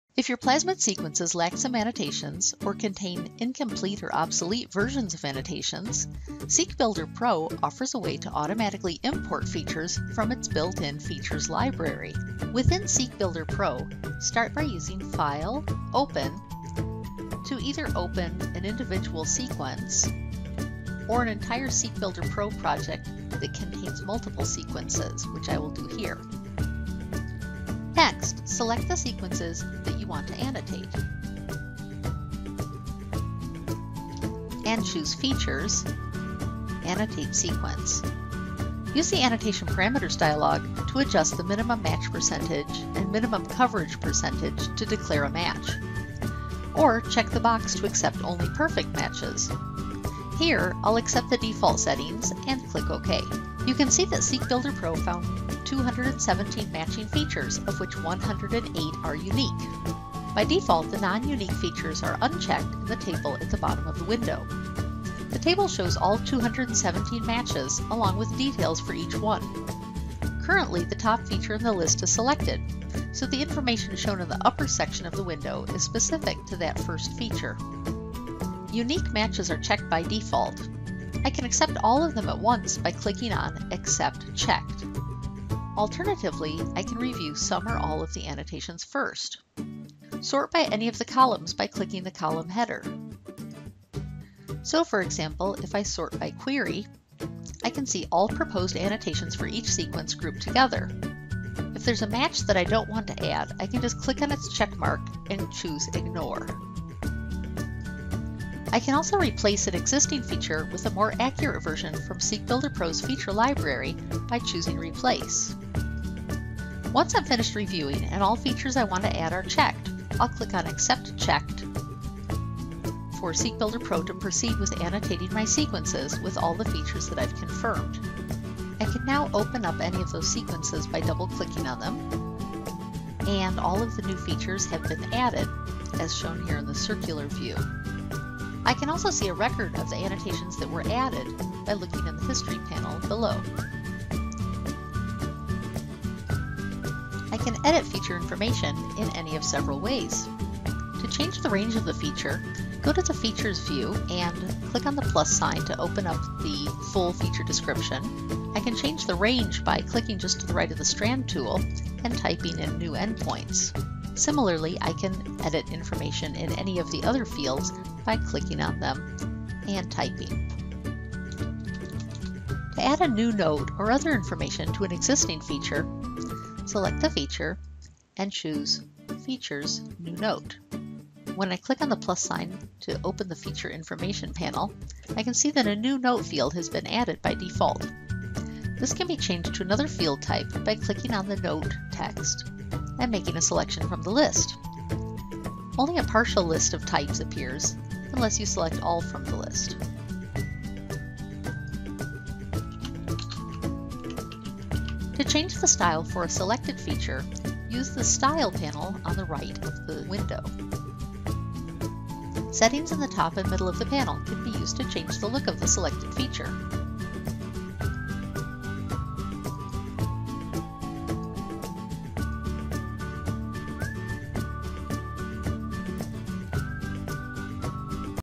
DNASTAR - Automated Plasmid Annotation
Plasmid auto-annotation in SeqBuilder Pro allows you to accurately and automatically annotate your sequences using a carefully curated database of features. Simply select your sequences and SeqBuilder Pro will provide you with a list of matched features for your consideration.

Quickly and accurately annotate your sequences — either one at a time or as a large batch.

Define the minimum match percentage and coverage percentage required for features to be considered a match, or choose to only find perfect matches.

Easily replace existing features with more accurate versions from the feature library.

Customize the installed database of annotations by adding your own features, if desired.